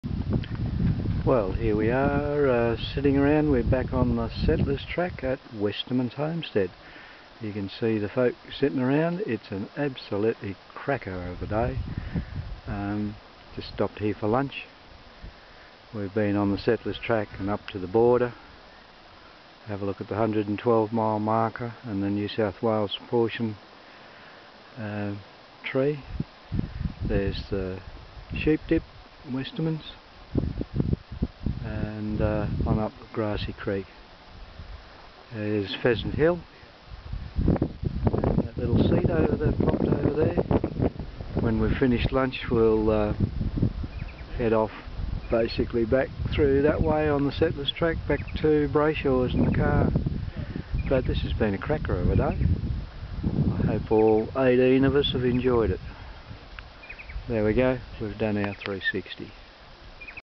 2010 12 28 The Settlers Track + extras
A gentle ramble around The Settlers Track in the southern ACT, with extensions to the border to see the 112M marker and the NSW portion reference tree